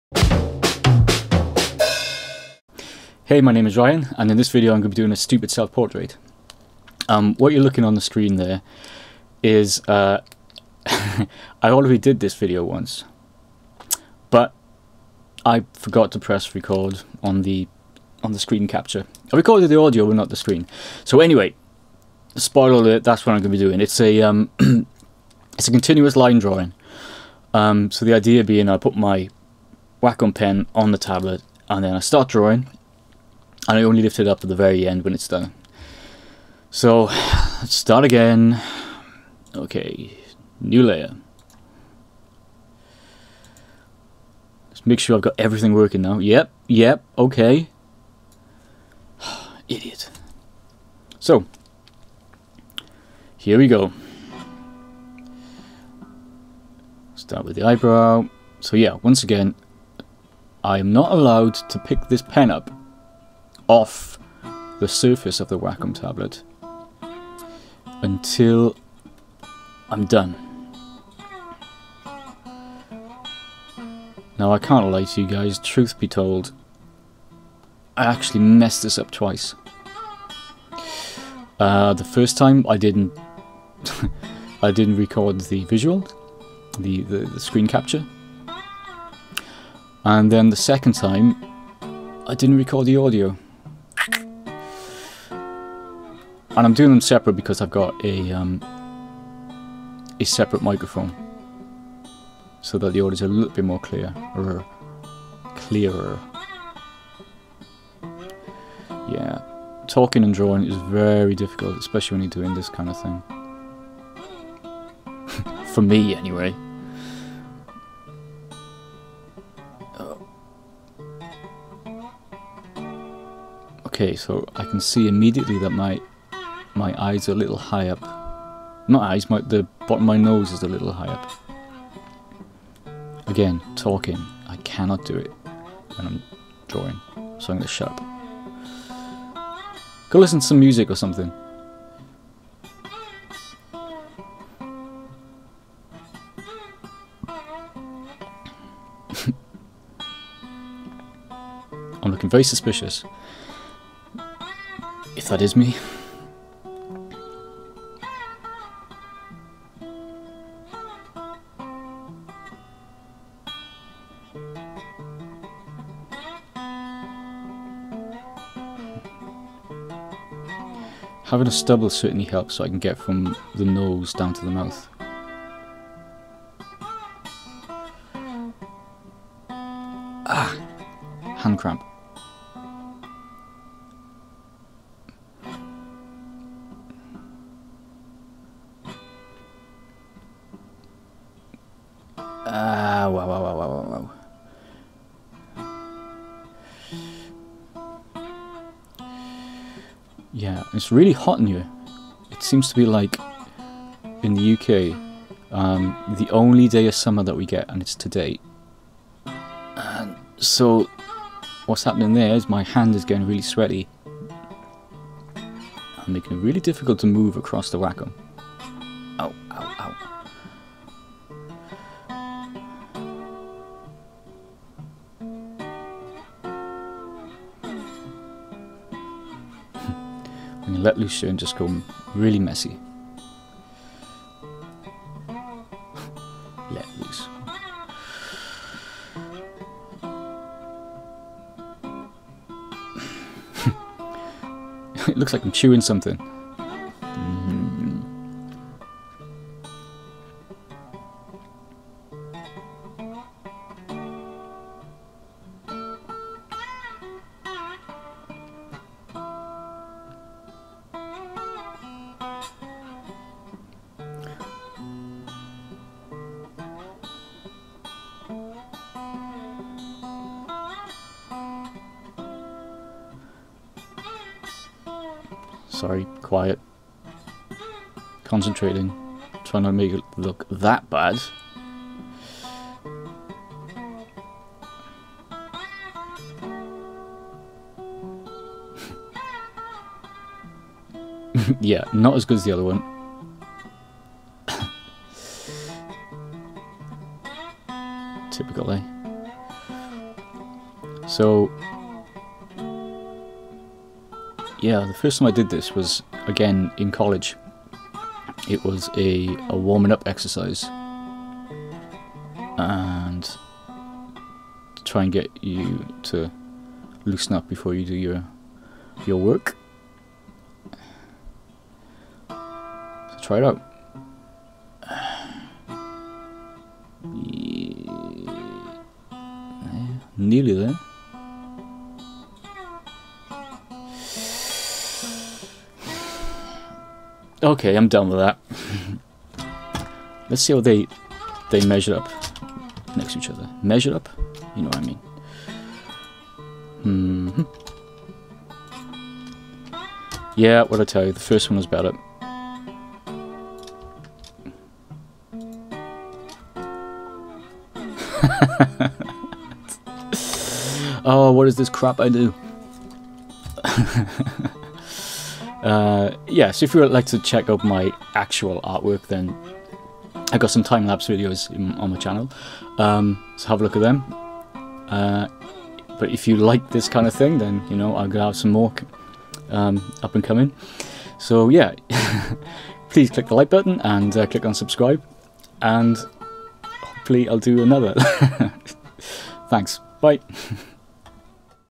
Stupid Portrait - Continuous Single Line Drawing
In this instalment of wonderfully and purposely bad portrait drawings, I draw a self portrait without taking the pen from the paper (Or wacom). I think it's called a continuous line drawing. Hand cramps, yes. Sweaty hands, yes. Aweseome drawing, no. Enjoy!